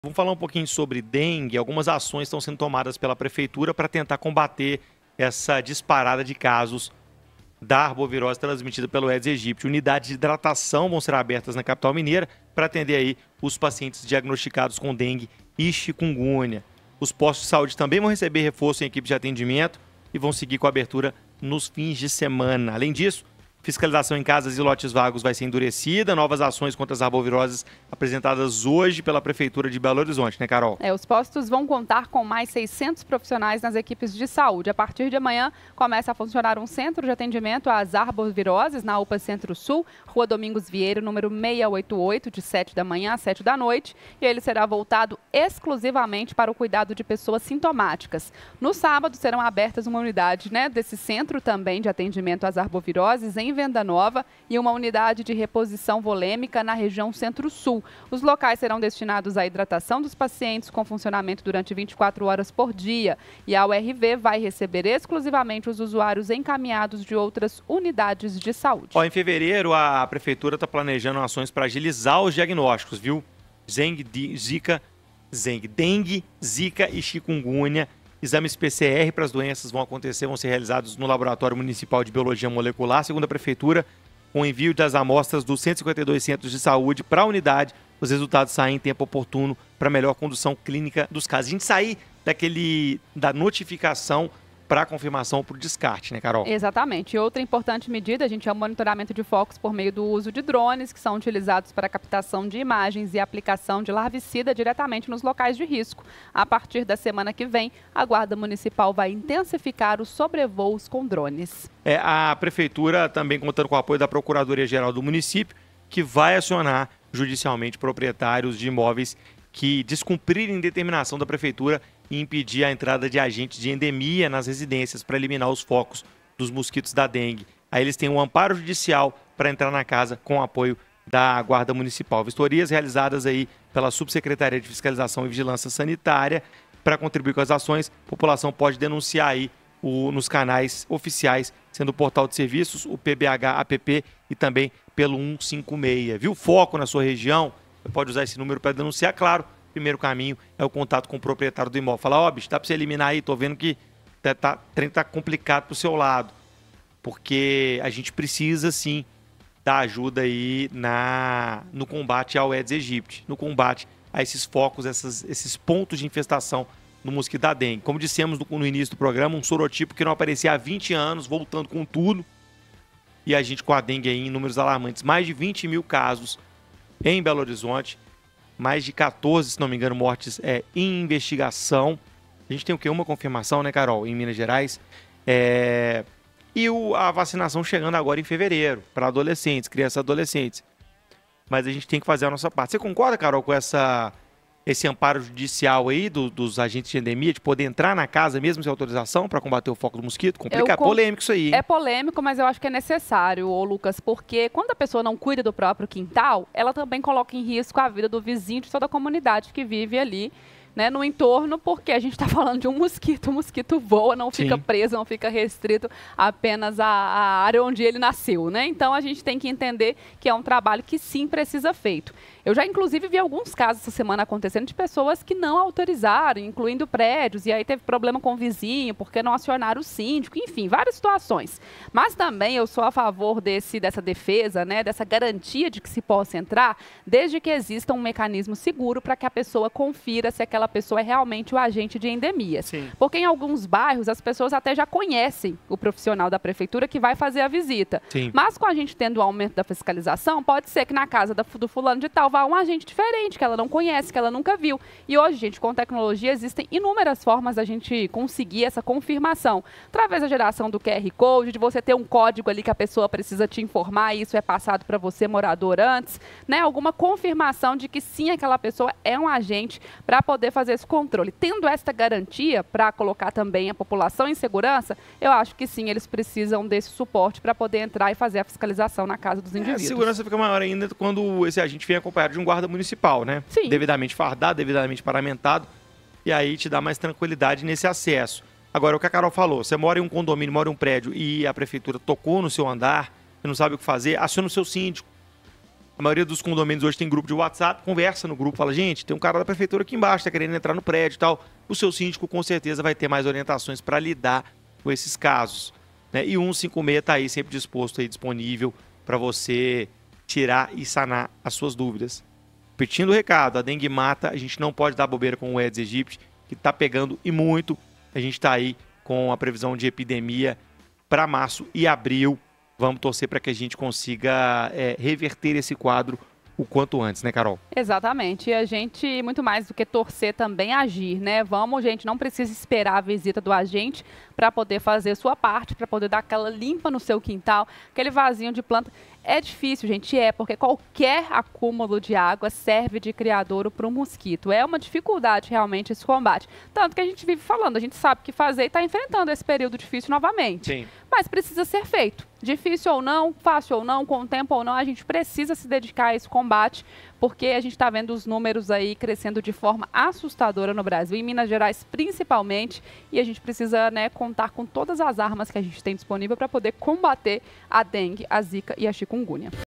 SOROTIPO que não circulava há 20 ANOS PROVOCA EXPLOSÃO de CASOS de DENGUE em BH
Unidades de hidratação serão abertas em Belo Horizonte para atender os pacientes com suspeita de dengue ou chikungunya. Postos de saúde também terão reforço de equipes de atendimento, e vão seguir com a abertura nos finais de semana. Além disso, a fiscalização em casas e lotes vagos será endurecida As novas ações contra as arboviroses foram apresentadas nesta quarta-feira pela Prefeitura de Belo Horizonte (PBH).

A grande preocupação das autoridades de saúde da capital mineira é com a circulação da dengue tipo 2, o que não acontecia há 20 anos. O vírus vem provocando uma escalada de infectados. Os dados mais recentes de 2024 mostram 947 casos confirmados de dengue e 59 de Chikungunya em BH.